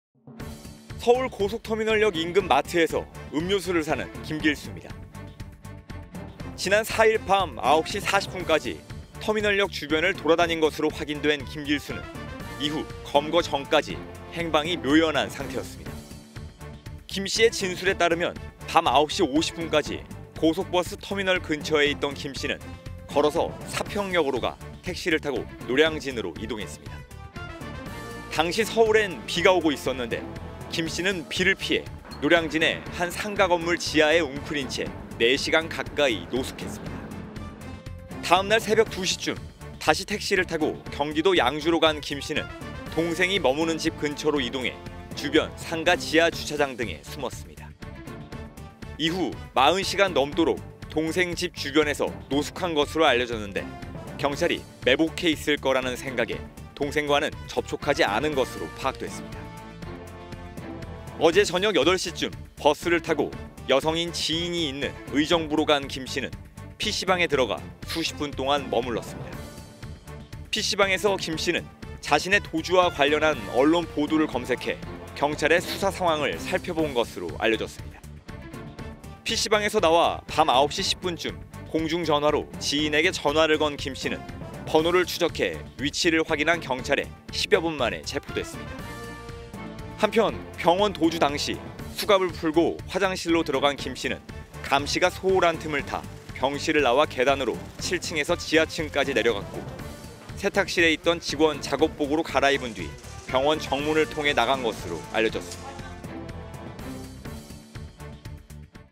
[자막뉴스] 고속터미널 이후 사라진 김길수, 어디서 뭐 했나 했더니? | 이슈픽
서울과 경기 일대를 종횡무진 헤집고 다닌 김길수는 고속터미널역 주변에서 모습이 포착되고선 검거 전까지 행적이 묘연한 상태였습니다.

붙잡힌 김 씨의 진술로 이후 동선이 알려졌는데, 그동안 대담한 도주 행각과는 달리 경찰의 추적을 피하려고 곳곳에서 노숙을 한 것으로 드러났습니다.